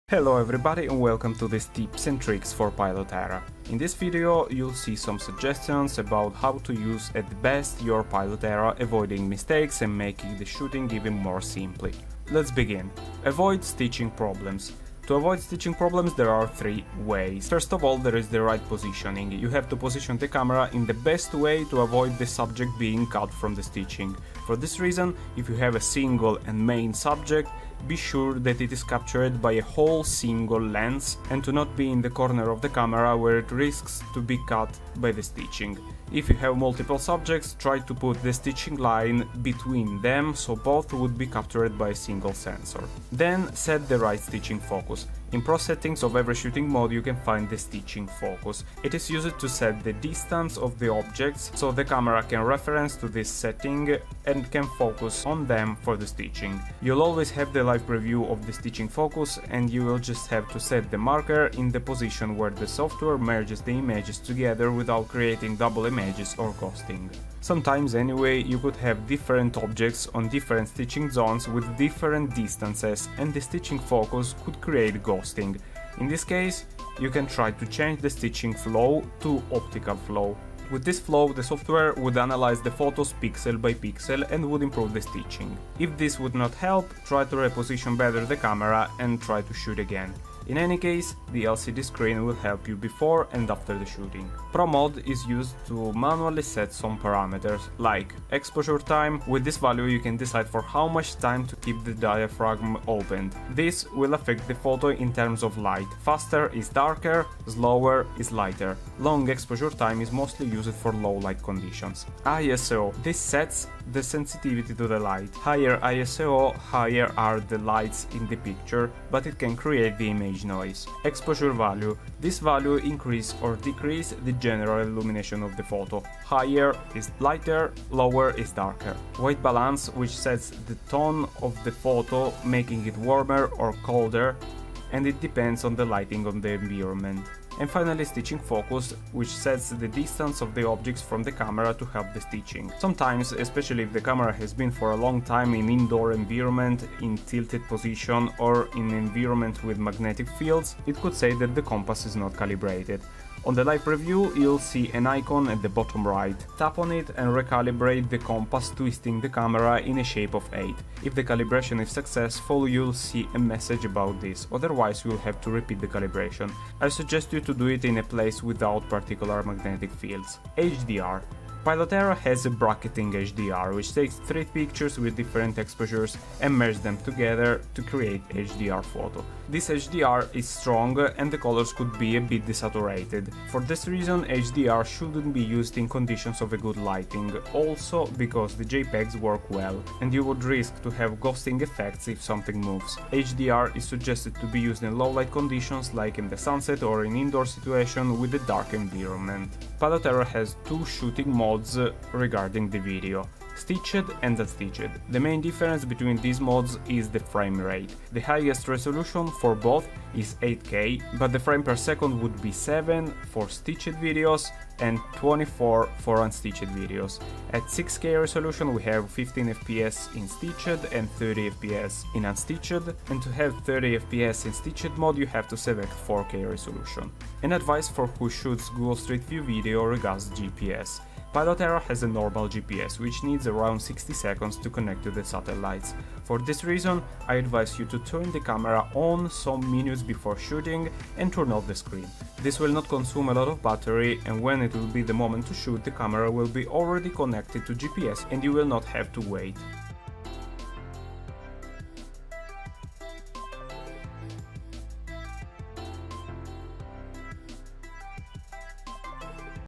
How to Use Pilot Era Tips & Tricks
Pilot Era: The first true all-in-one 8K 360 VR camera. 8K video real-time stitching, smart OS, standalone 4K live streaming,  Google Street View Ready camera.